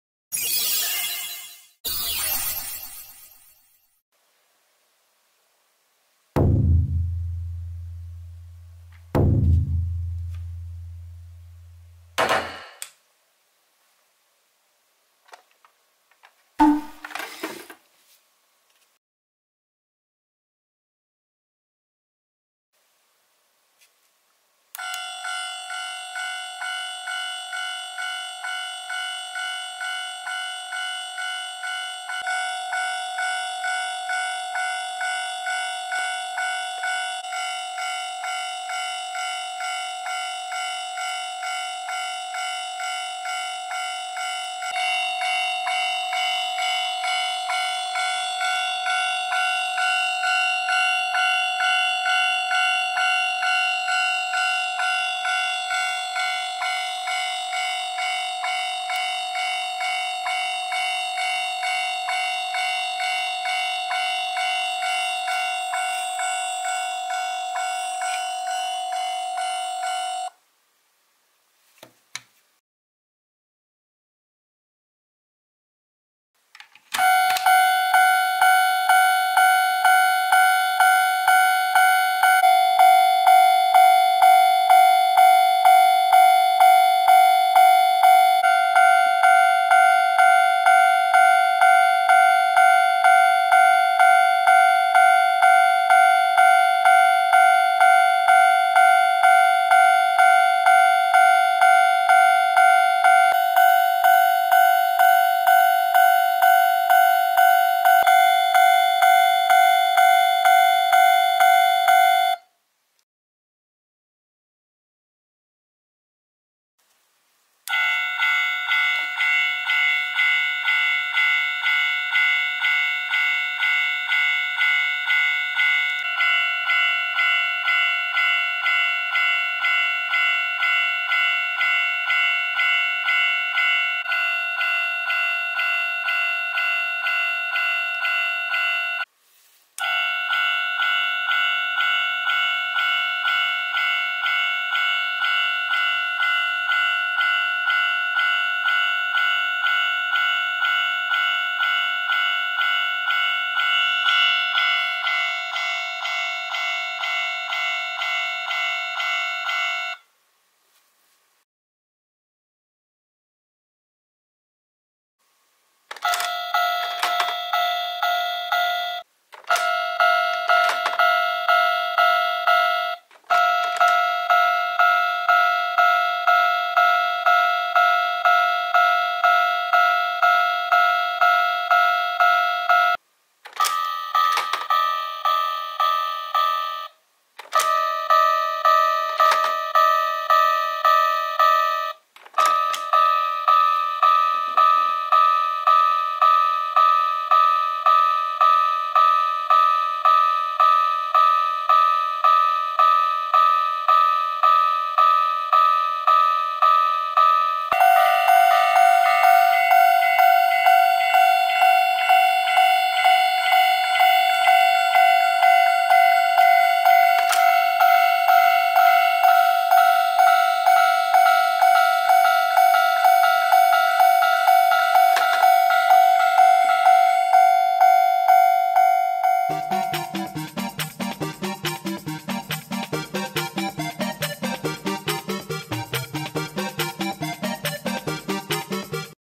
踏切音比較 Comparison of railroad crossing sounds
踏切のおもちゃの音を比較してみました！
①字幕有り スーパーのりものデラックス　ふみきりのヒミツ！　付録の踏切
②トミカタウン　サウンドライトふみきり
③踏切コレクション2
④プラレール　J-19サウンドふみきりセット

あなたはどの音が好き？